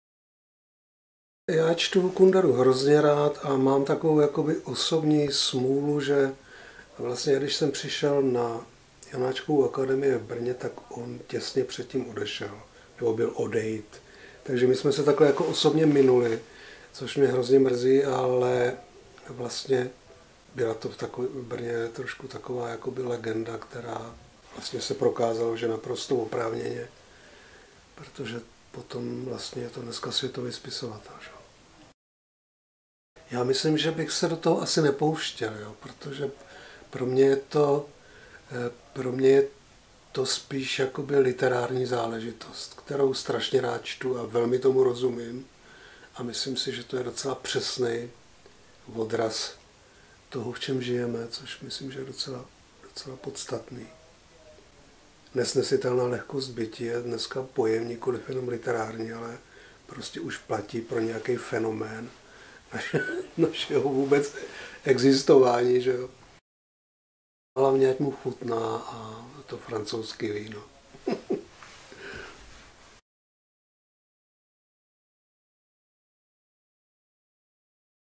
Miroslav Krobot o Milanu Kunderovi
Jak vnímá dílo Milana Kundery režisér Miroslav Krobot? Podívejte se na rozhovor. 1. dubna 2019 se můžete těšit na první část Četby na pokračování z Kunderova románu Nesmrtelnost. Více zde ►rozhl.as/Kundera